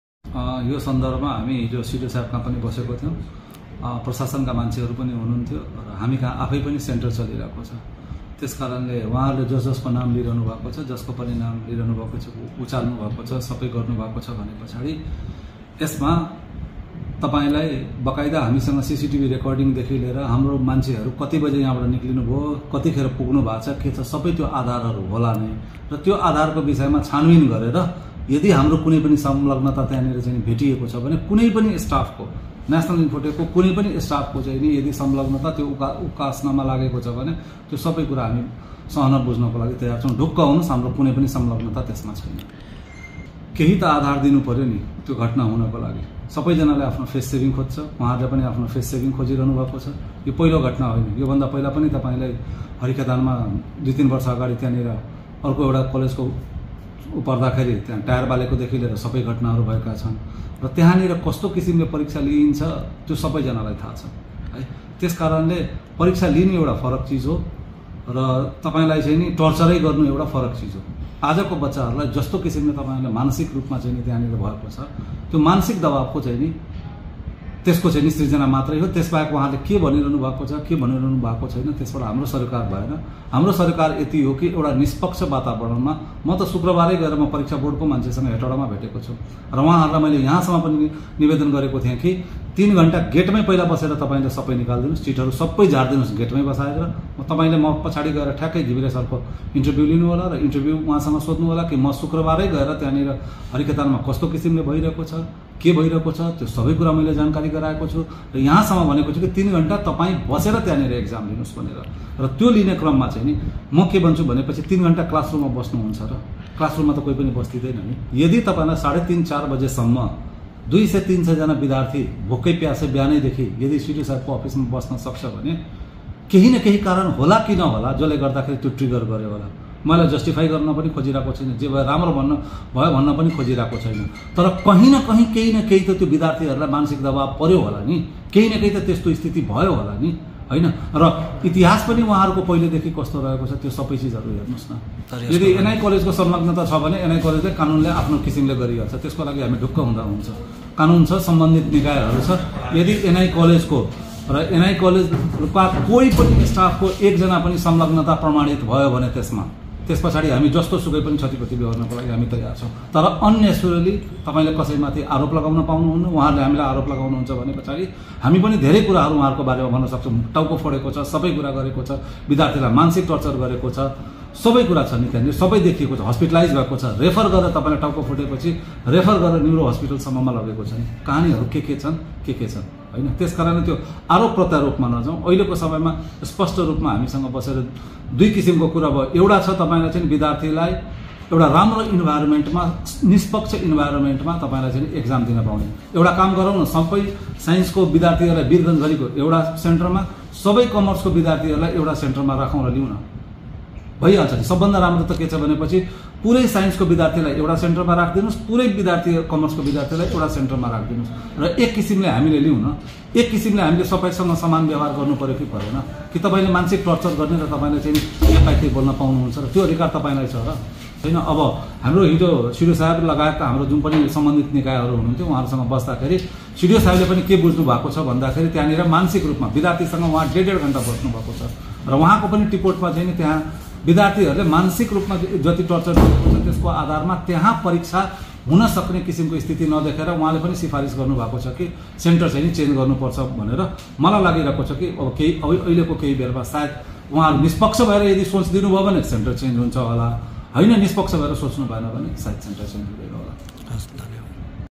हरिखेतान क्याम्पस तोडफोडमा मेराे कुनै कर्मचारीको संलग्नता छैन : सञ्चालक मनिष सिंह
हरिखेतान क्याम्पस तोडफोडमा नेशनल ईन्फाेटेक कलेजका कुनै कर्मचारीको संलग्नता भेटिए क्षतिपूर्ति बेहोर्न तयार छौं : सञ्चालक मनिष सिंह